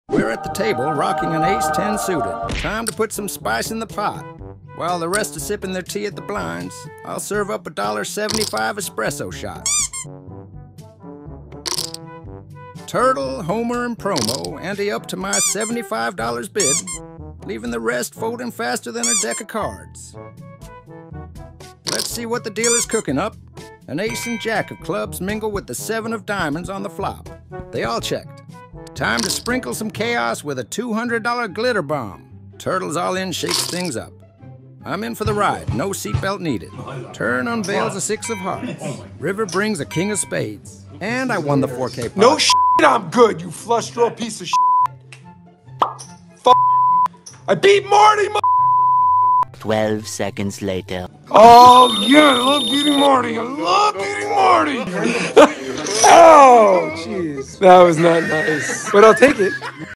Trash Talk Gone Wrong! But i still won 4k POT!
Title: "Epic Fail: Trash Talk Gone Terribly Wrong in Poker Game!"Description: In this hilarious yet cringe-worthy video, watch as Kravitz attempts to assert dominance through some serious trash talk against Turtle. Little did they he know, it was about to backfire in the most spectacular fashion! From confident boasts to epic fails, this video has it all. Get ready for some jaw-dropping moments, unexpected twists, and plenty of laughs as Kravitz learns a valuable lesson in humility. Don't miss out on witnessing the epic fail that will leave you shaking your head and laughing out loud!